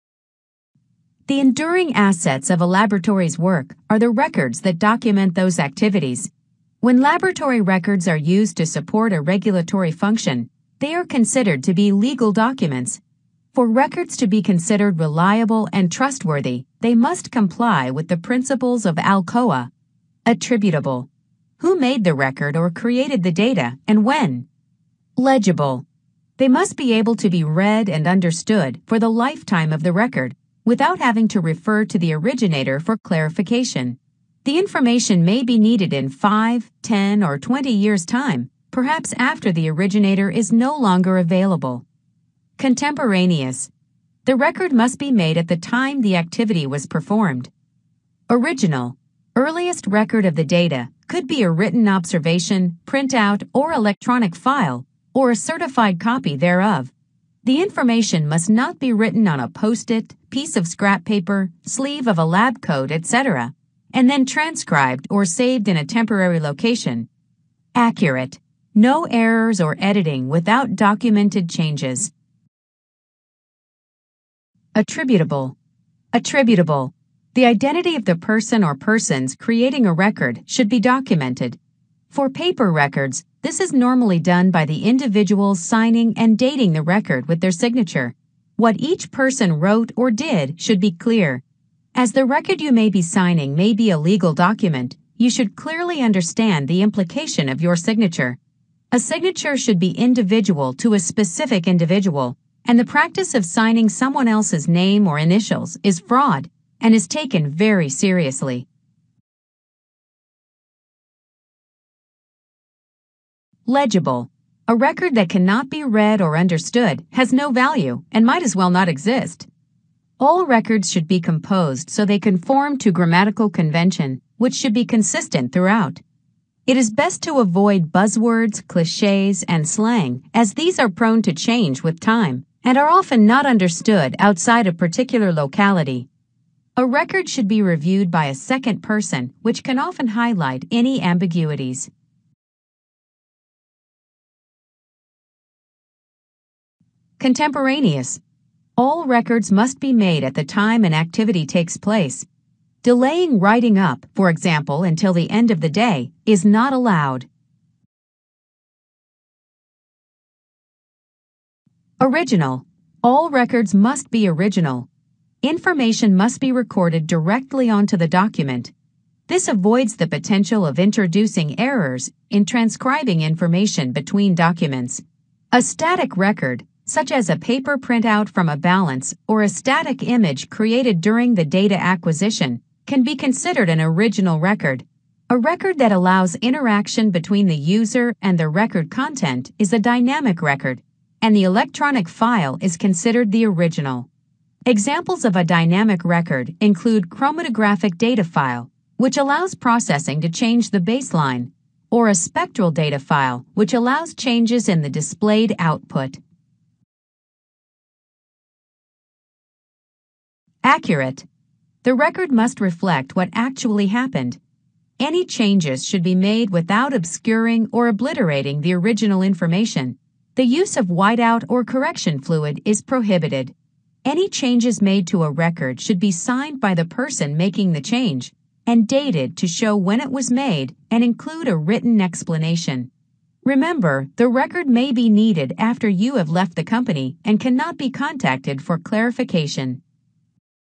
Data Integrity - ALCOA
The five main principles of data integrity are:

A: Attributable
L: Legible
C: Conemporaneous
O: Original
A: Accurate